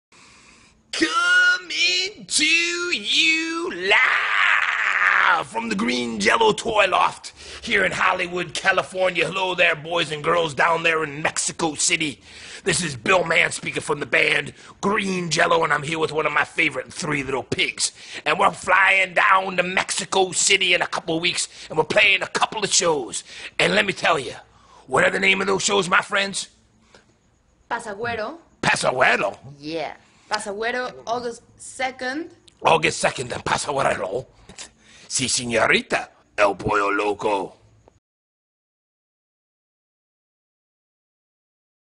Green Jello en México - 2 de Agosto 9:00pm en Pasagüero
Sábado 2 de Agosto a las 9:00pm Green Jello en Pasaguero

Preventa: $120
Día del evento: $180